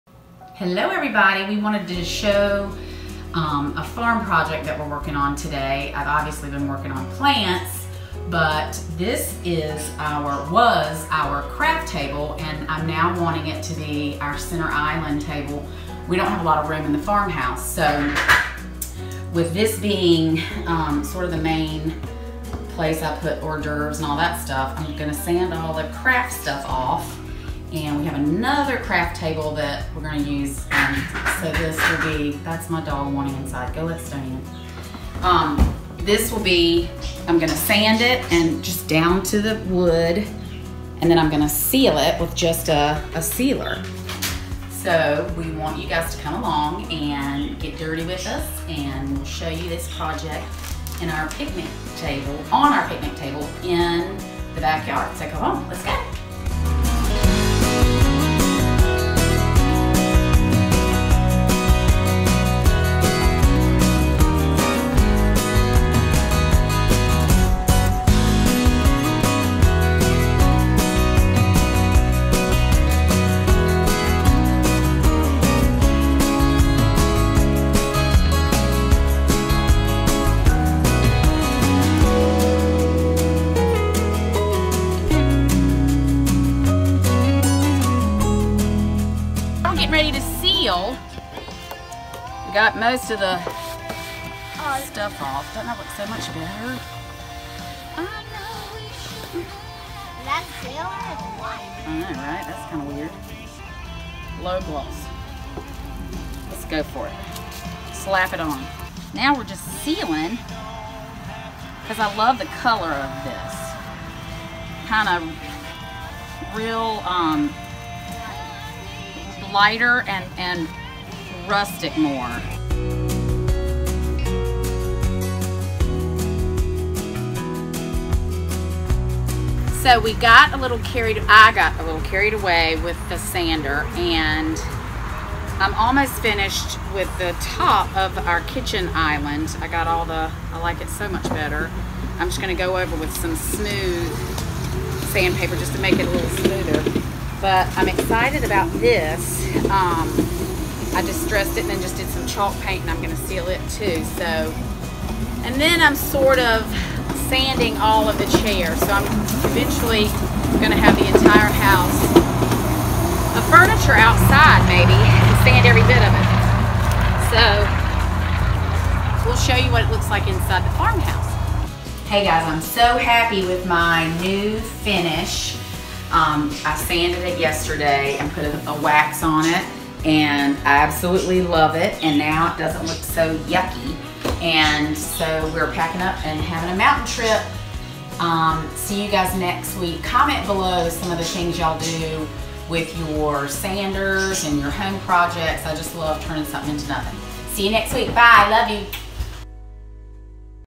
CRAFT TABLE REFINISH DIY!
We love crafting and doing things ourselves. We took our craft table and sanded it then refinished it! We love the way it turned out. What do you think?

Follow me and Skylynn on Social Media!